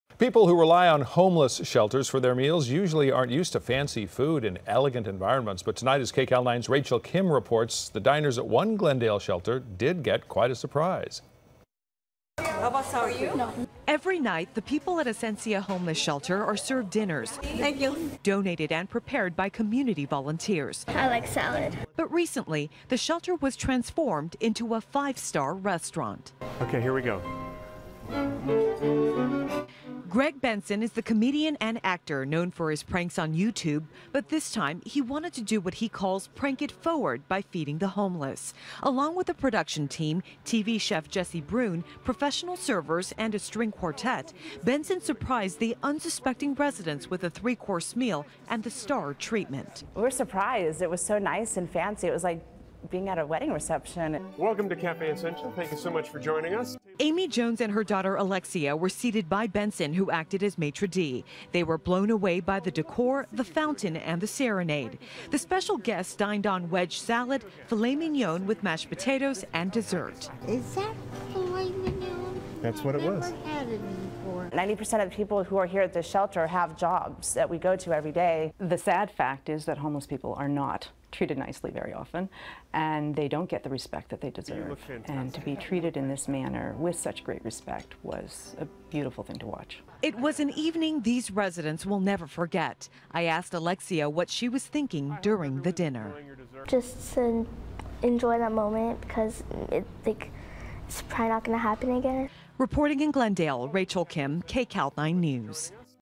Comedian Helps To Transform Glendale Homeless Shelter Into Five-Star Restaurant
Comedian Greg Benson, the man behind the popular "Cellphone Crashing"  videos on YouTube, helped to transform a Glendale homeless shelter into a  five-star restaurant for April Fool's Day. KCAL9's Rachel Kim reports.